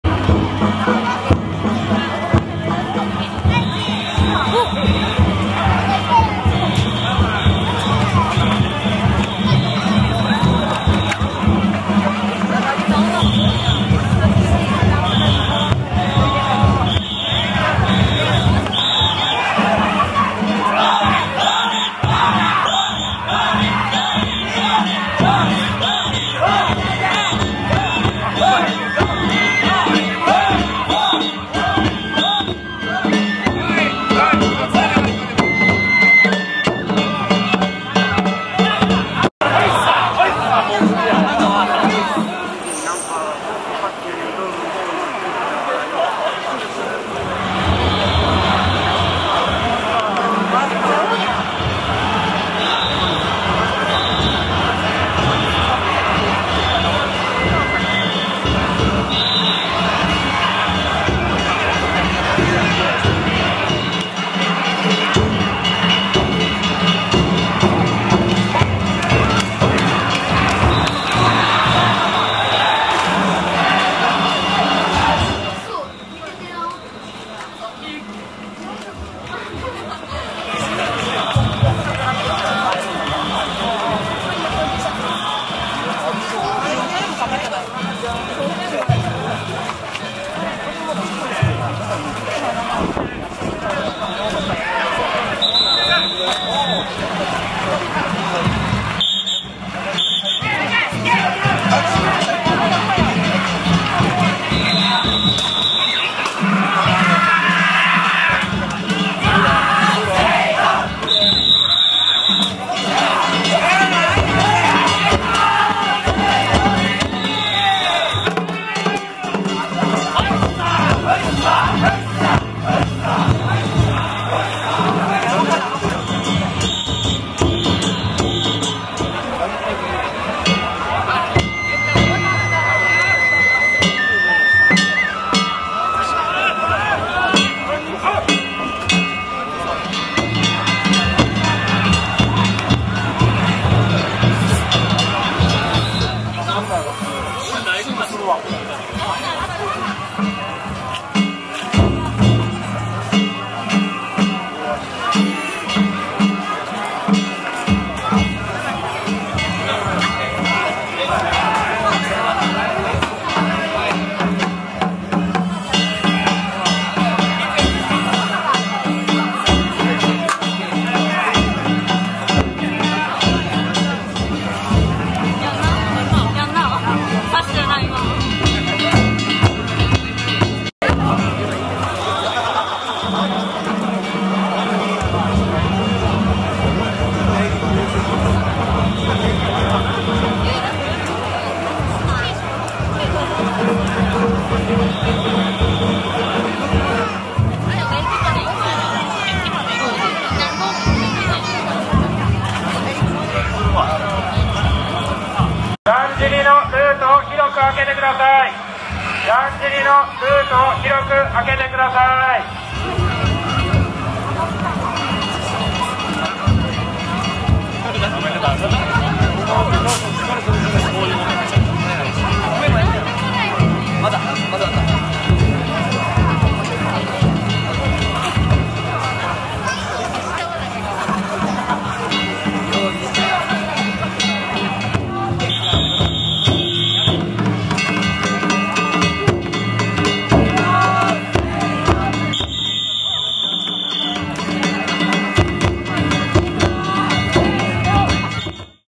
中北町 岸和田だんじり祭＿2022年
威勢のいいスピード感ゆたかなやり回しで知られる中北町の曳行光景。夜の灯入曳行も情緒がある。 岸和田だんじり祭、2022年9月撮影。OMD-EM1mark2